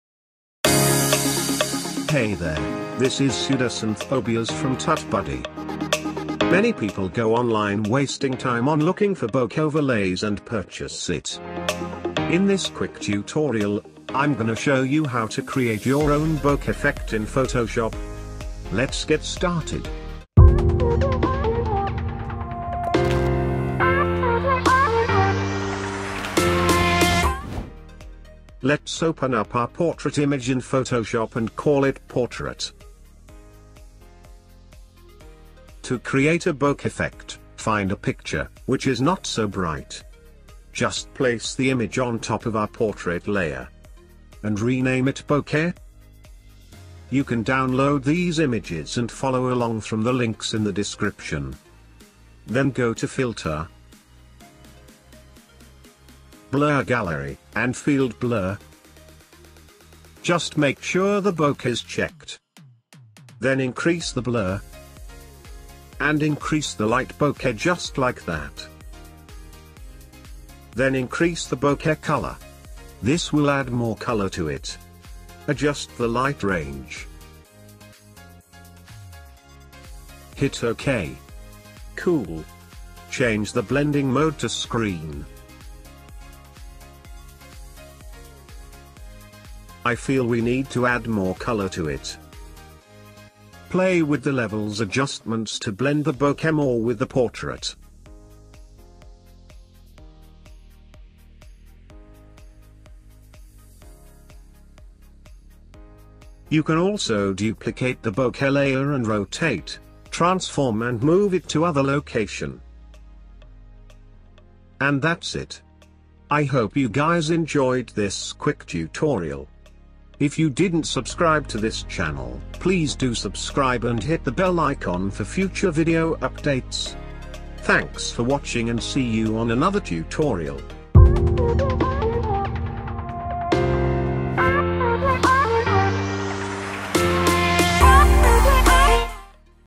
Create BOKEH OVERLAYS in PHOTOSHOP
In this video, we are going to create our own bokeh overlays.